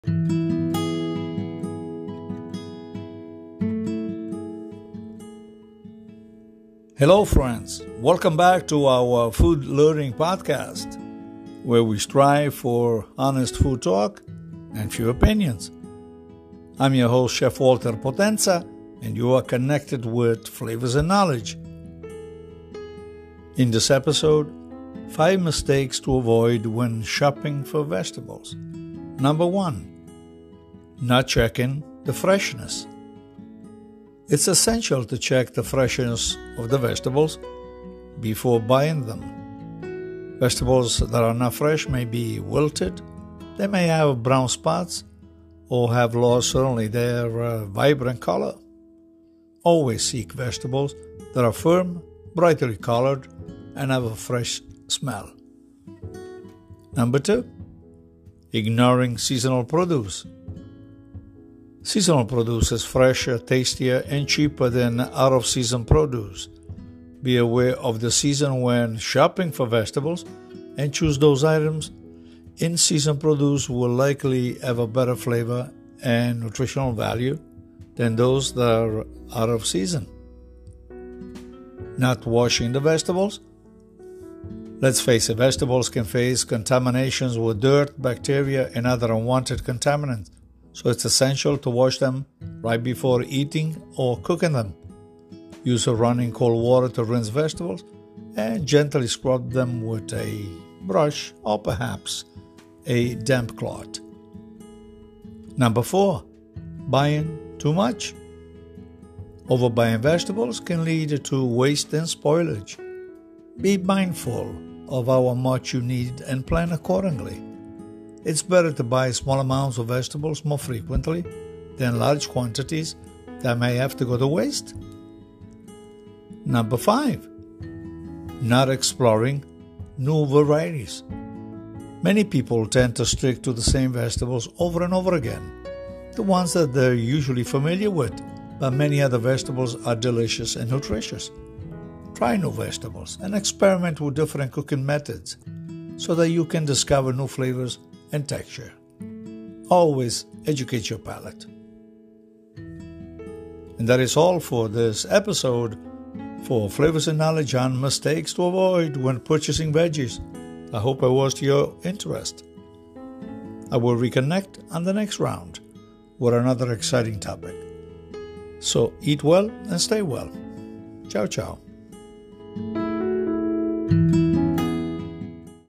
{165} 5 MISTAKES WHEN BUYING VEGGIES
Chef Walters suggestions on mistakes to avoid when purchasing vegetables.